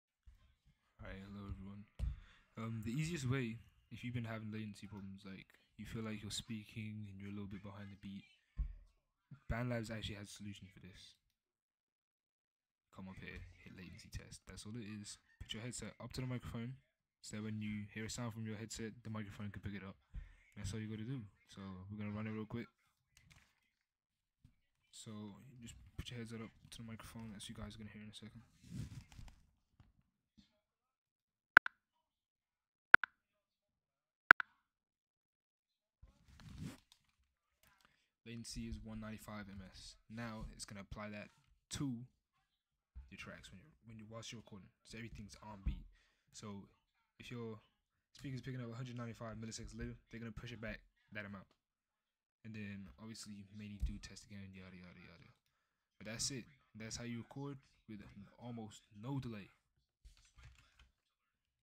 How to fix recording latency (bandlab)
This will help out in bandlab, this is pretty much all you can do before buying hardware to solve the issue but I hope this helps!

likes and subs much appreciated